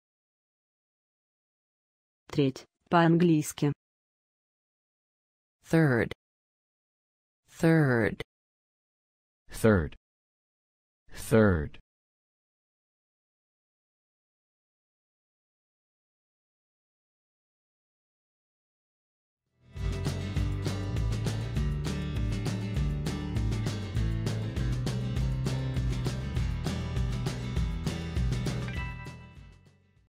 Треть по-английски
Как правильно сказать Треть по-английски? Слушай вместе с нами.
А как будет Треть в переводе с русского на английский? Говори, как произносят американцы. И еще, как правильно написать слово Third?

Спасибо!